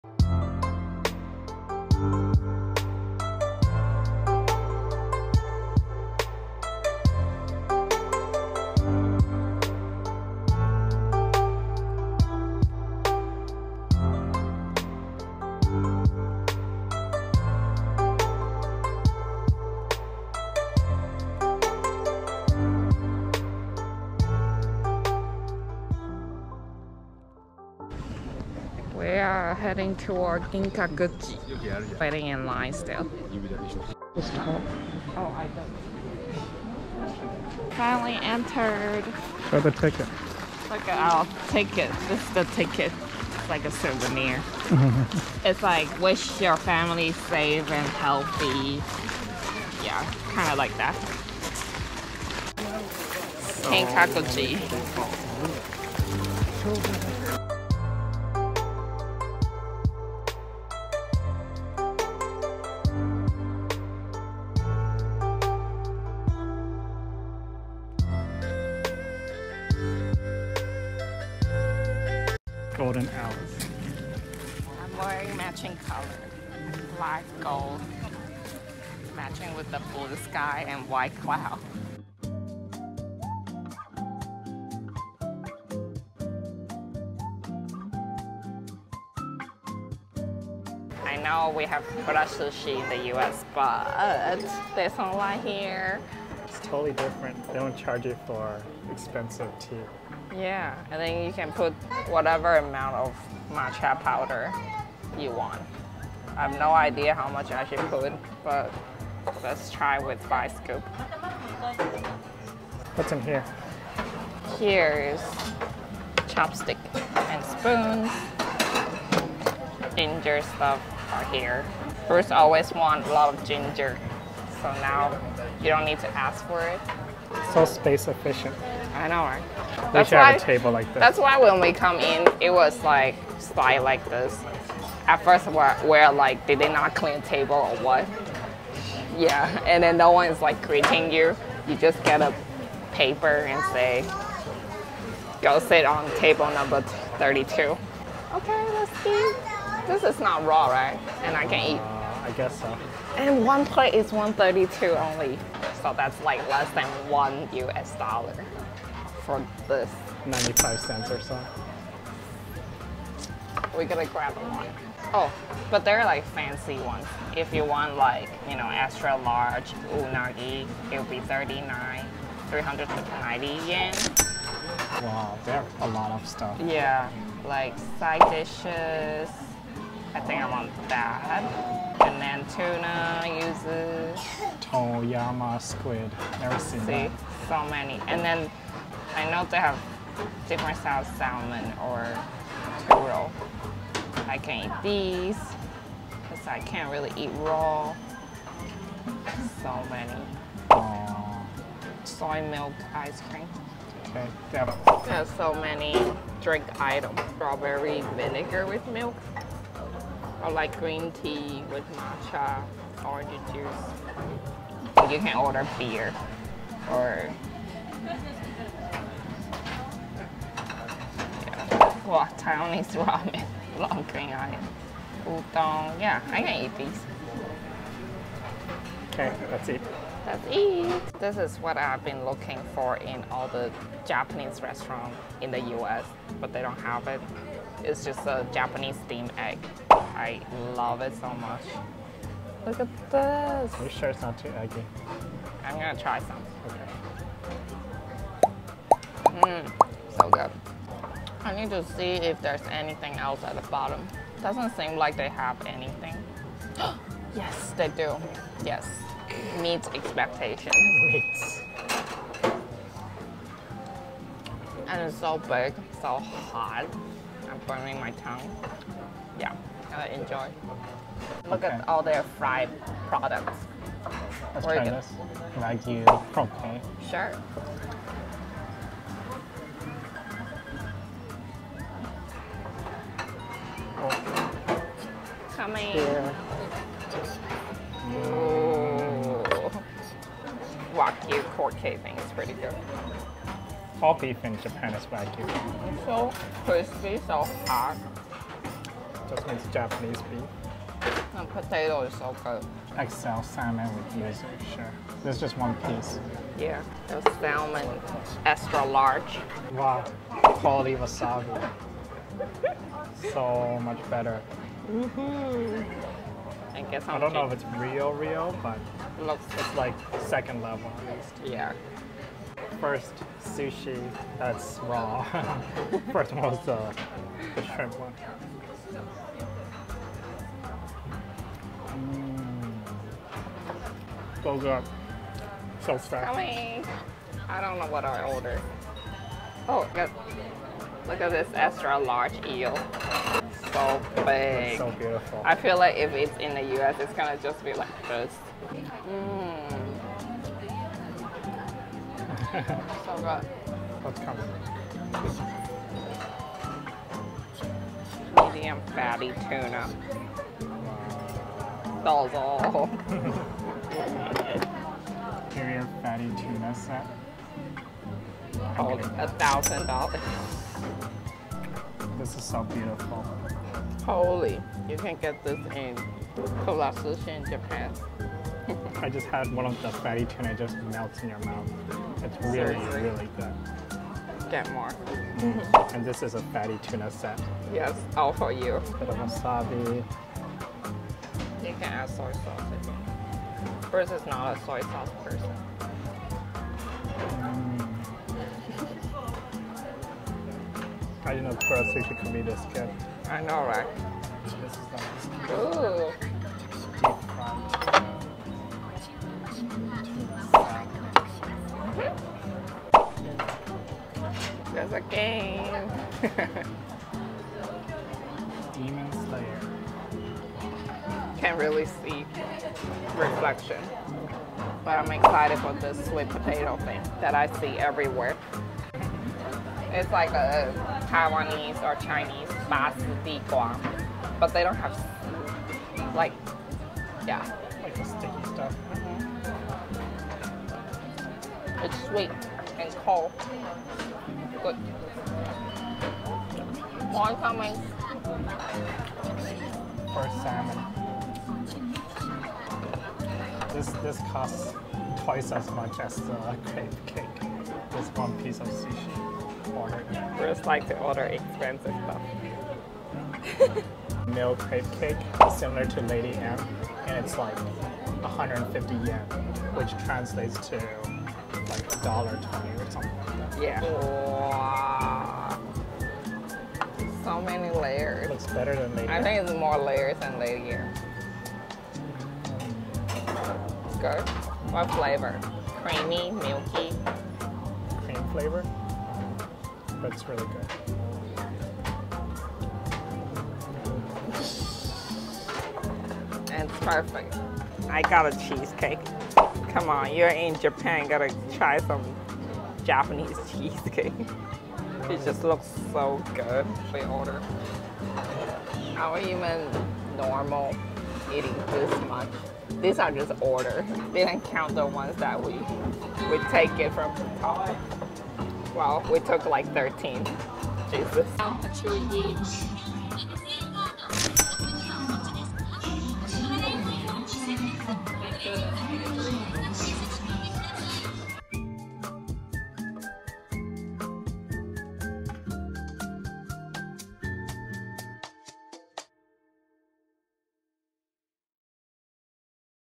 Must Eat Japan Conveyor Belt Sushi 🍣 $1 Dollar Sushi Review ☀ Visited Japan Golden Pavilion
After visiting Gingakuji, we went to one of the most popular conveyor belt sushi restaurants in Japan - Kura Sushi. OMG the Japanese Kura sushi is SO DIFFERENT than the ones in the USA! Each dish is only less than 1 USD, and the quality is so much better!